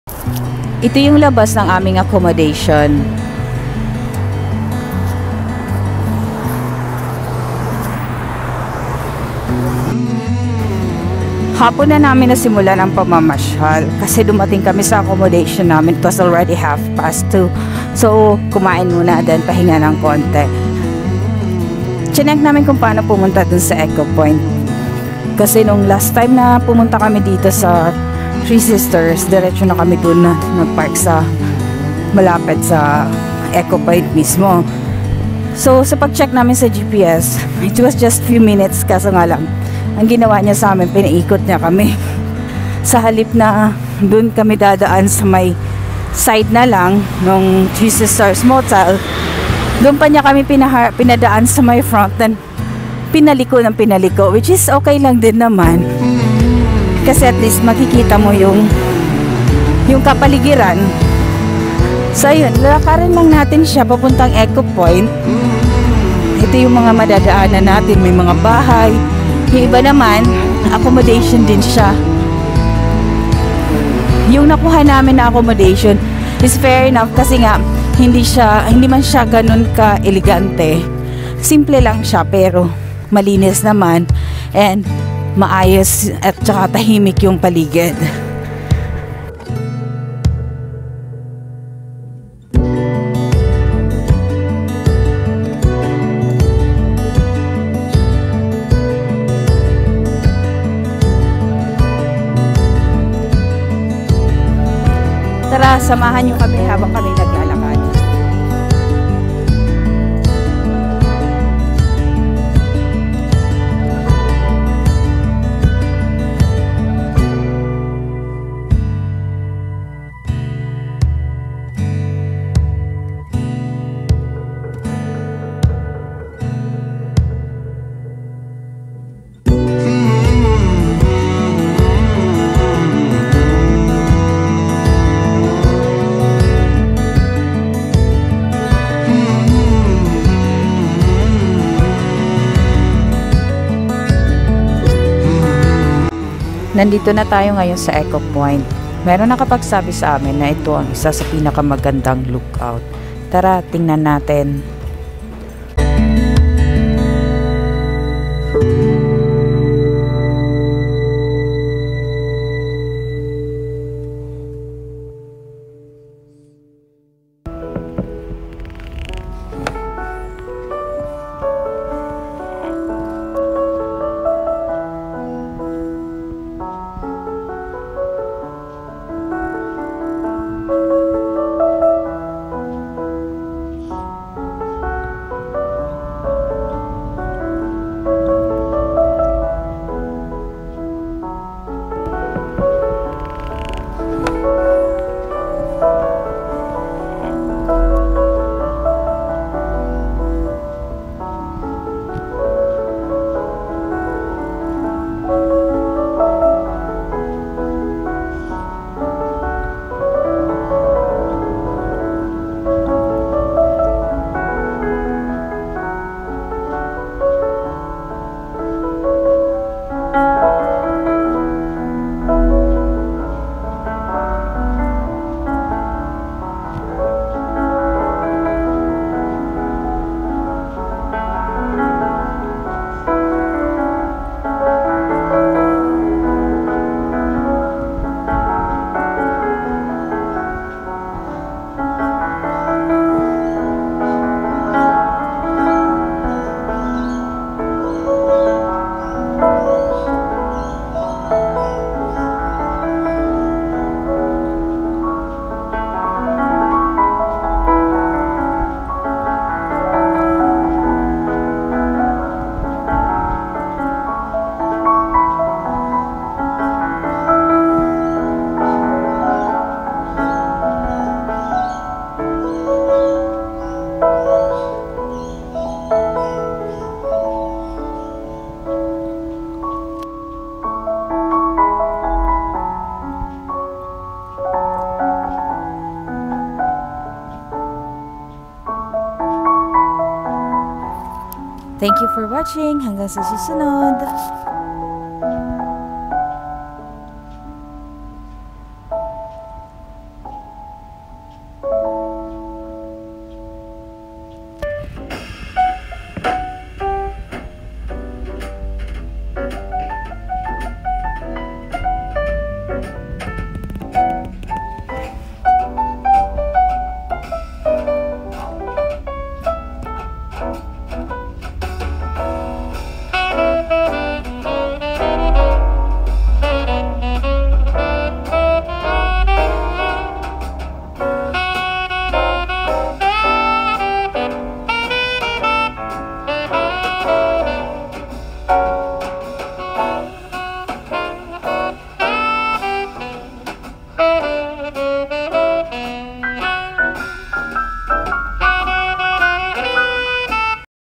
Three Sisters Blue Mountains | Echo Point
The Three Sisters is the Blue Mountains’ most spectacular landmark. Located at Echo Point Katoomba, around 2.5 kilometres from the Great Western Highway. An unusual rock formation representing three sisters who according to Aboriginal legend were turned to stone.